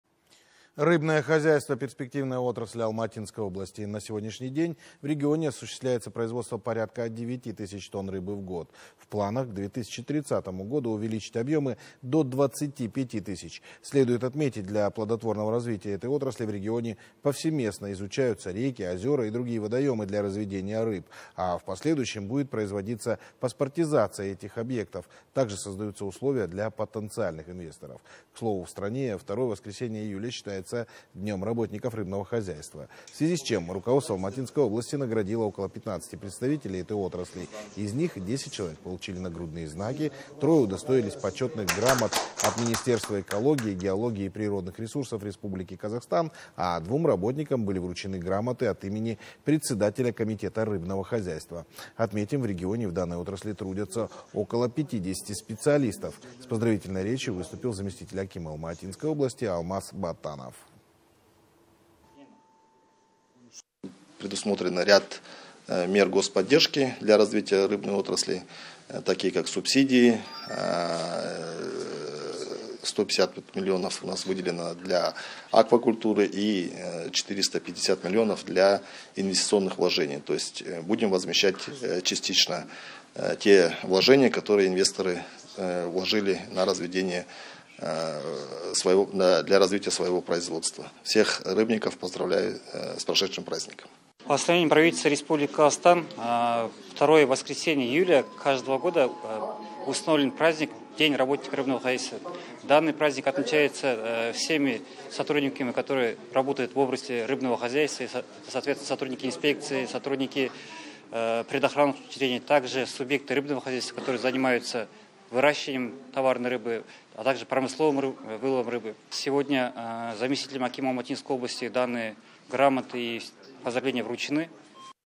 Рыбное хозяйство - перспективная отрасль Алматинской области
Рыбное хозяйство - перспективная отрасль Алматинской области. На сегодняшний день в регионе осуществляется производство порядка девяти тысяч тонн рыбы в год. В планах к 2030-му году увеличить объемы до двадцати пяти тысяч. Следует отметить, для плодотворного развития этой отрасли в регионе повсеместно изучаются реки, озера и другие водоемы для разведения рыб. А в последующем будет производиться паспортизация этих объектов.